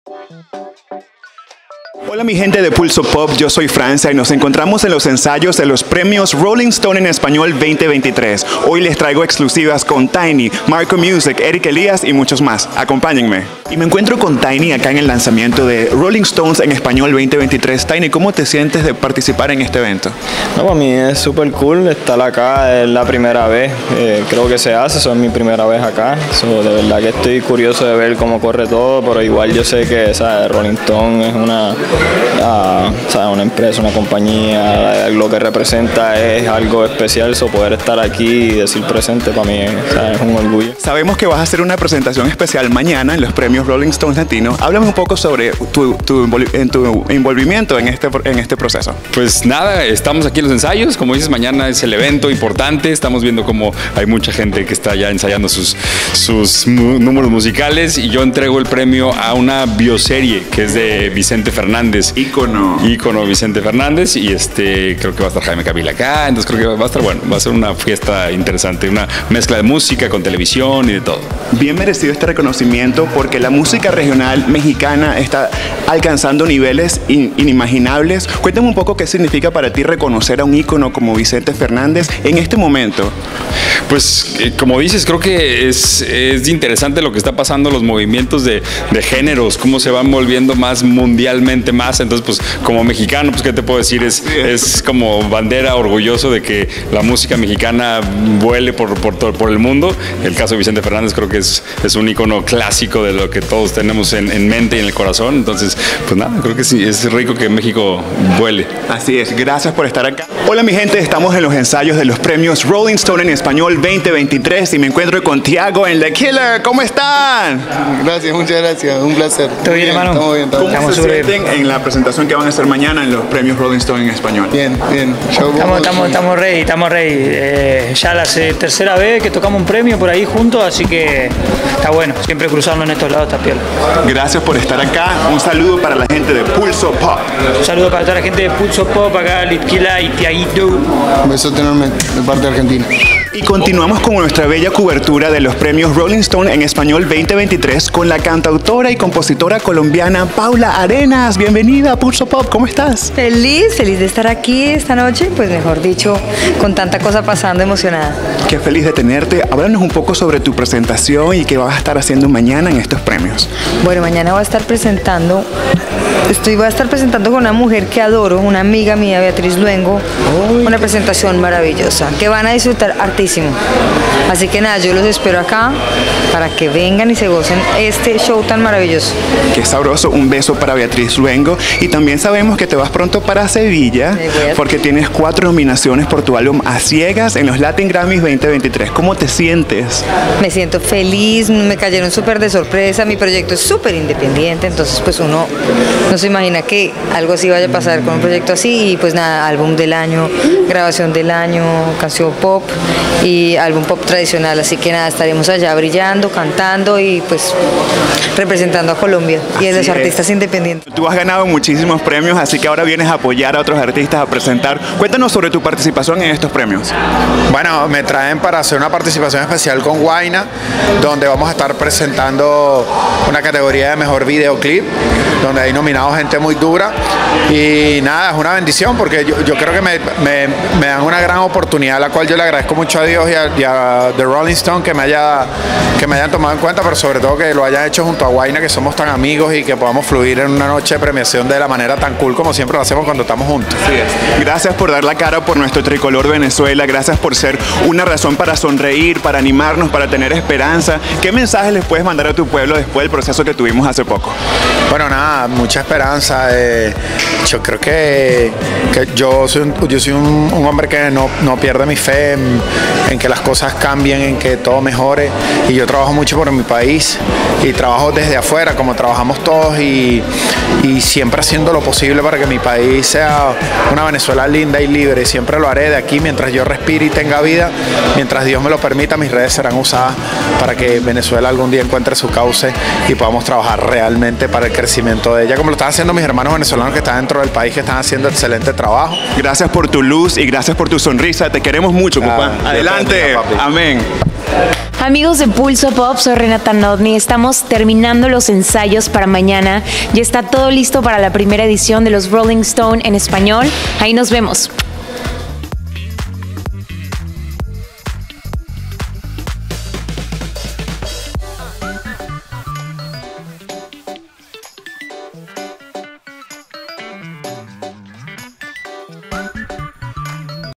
PREMIOS ROLLING STONE: Entrevistas backstage TIAGO PZK, LIT KILLAH, RENATA NOTNI & más
Estuvimos backstage desde los Premios Rolling Stone con @FranciaCanta ✨️